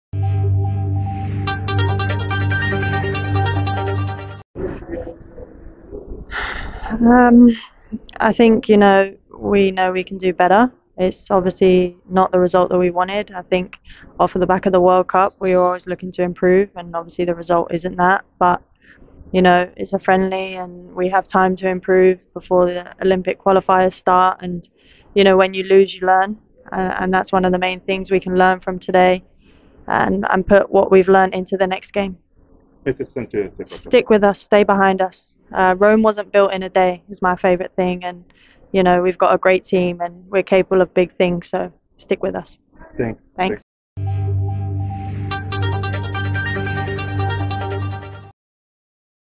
لاعبة توتنهام و المنتخب الوطني روزيلا بعد الهزيمة أمام زامبيا " هدفنا الألعاب الأولمبية"
لاعبة توتنهام و المنتخب الوطني روزيلا بعد الهزيمة أمام زامبيا " هدفنا
الألعاب الأولمبية"